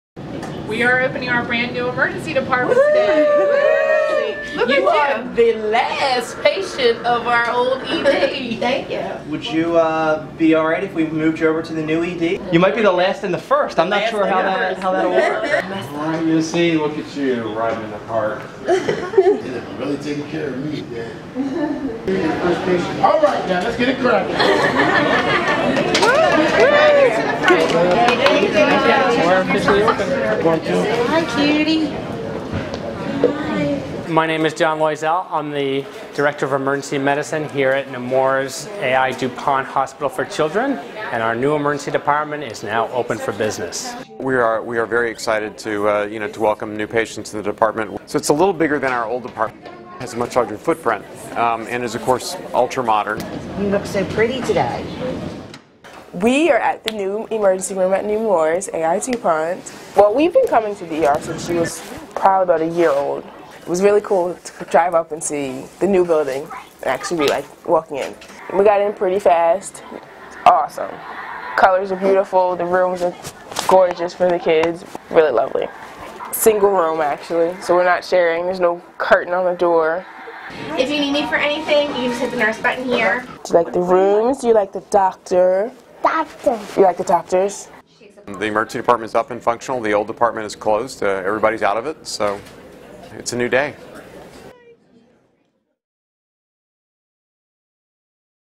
New Emergency Department Now Open at Nemours/Alfred I. duPont Hospital for Children
One week ago, we opened our state-of-the-art Emergency Department at duPont Hospital for Children. As Delaware's only Level 1 Pediatric Trauma Center, we've expanded from 24 to 44 treatment rooms, nearly doubling the hospital’s capacity to treat emergencies. This video give you a glimpse of that historic first day.